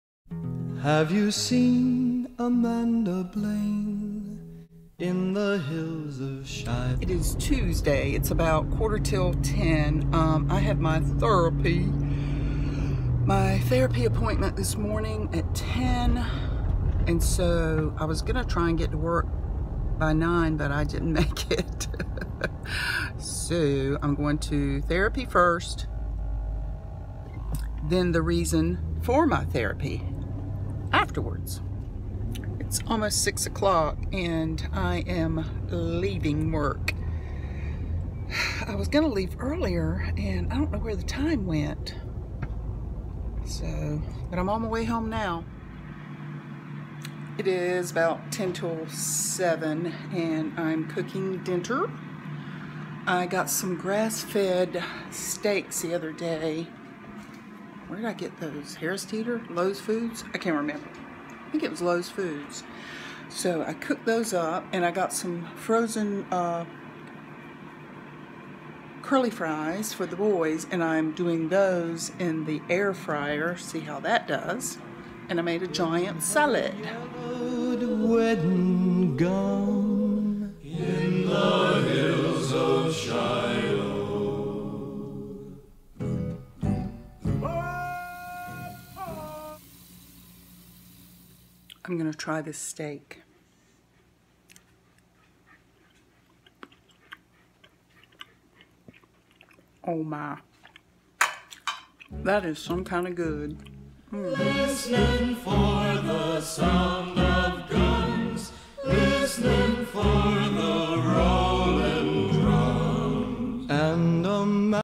Vlog 863 - March 5, 2019 ~ Got Some Grass Fed Steak First Time
Trying grass fed beef for first time